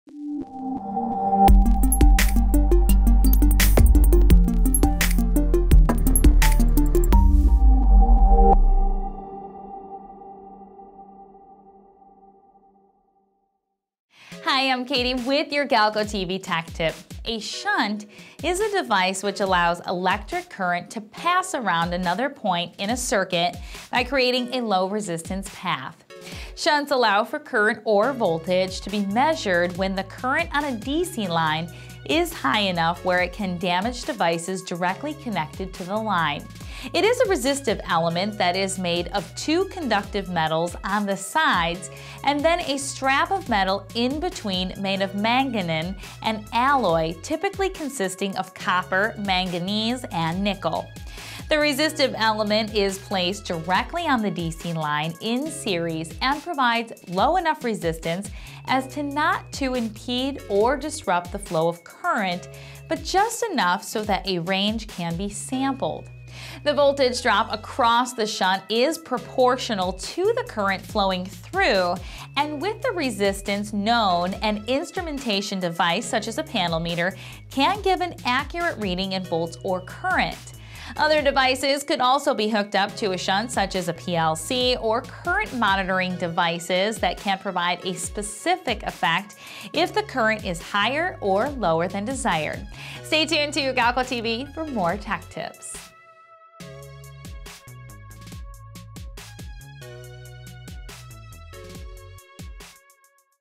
What is a Shunt? - A Galco TV Tech Tip
A shunt is a device which allows electric current to pass around another point in a circuit by creating a low resistance path. Shunts allow for current or voltage to be measured when the current on a DC line is high enough where it can damage devices directly connected to the line.  It is a resistive element that is made of two conductive metals on the sides and then a strap of metal in between made of manganin, an alloy typically consisting of copper, manganese and nickel.  The resistive element is placed directly on the DC line in series and provides low enough resistance as to not to impede or disrupt the flow of current, but just enough so that a range can be sampled.  The voltage drop across the shunt is proportional to the current flowing through and with the resistance known an instrumentation device such as a panel meter can give an accurate reading in volts or current.